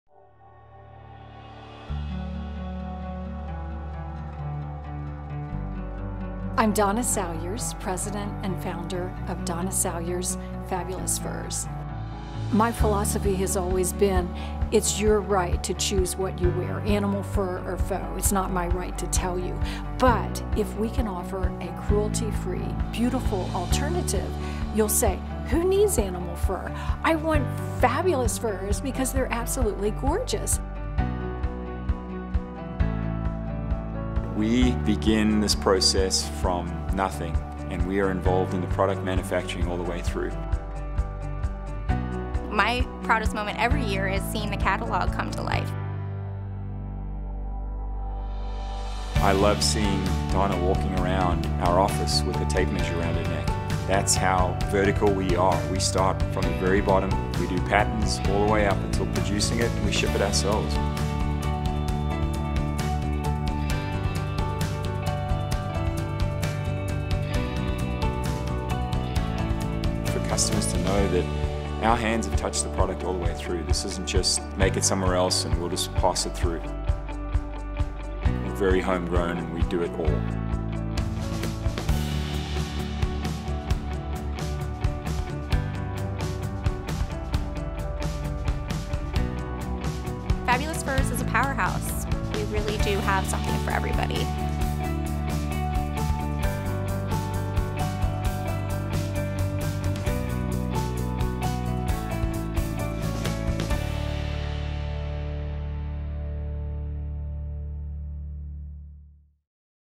Donna Salyers Fabulous-Furs: A Look Inside
Our brand began with a passion for animals, needle, thread, and the belief that cruelty-free fashion could be achieved. Nearly 30 years ago, after sewing a luxurious faux fur coat for herself, Donna Salyers designed a sewing kit so others could do the same. Success followed passion and excellence, and Donna ushered in an exciting new era for the faux fur industry.

At Donna Salyers Fabulous-Furs, we remain loyal to the true craftsmanship that goes into creating our products. Our fabrics and materials are carefully selected to ensure the finest luxury faux furs.